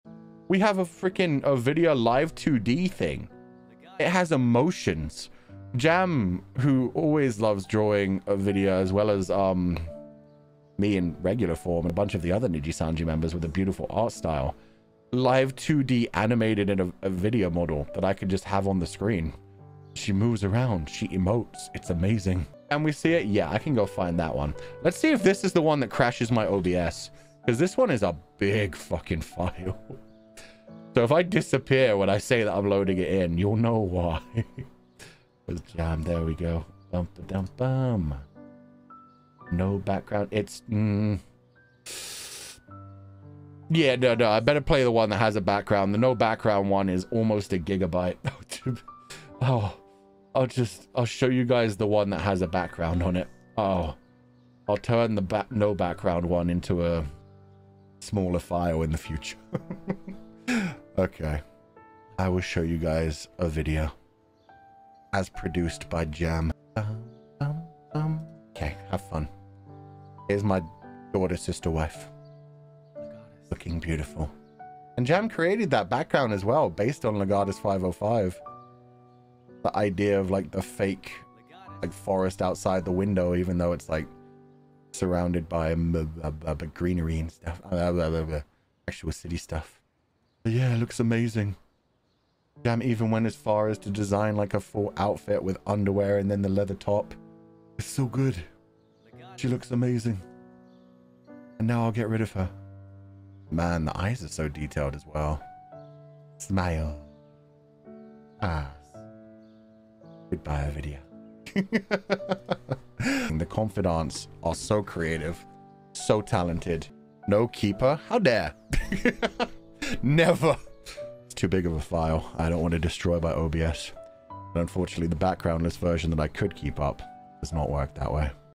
【彩虹社EN】粉絲慶祝Fulgur生日幫他畫出女兒兼妹妹兼老婆 還有精美的2Dlive動畫！！ 但是可惜Fulgur只能把她丟了【Fulgur Ovid】【Vtuber精華】
有任何對影片的建議請不吝指教～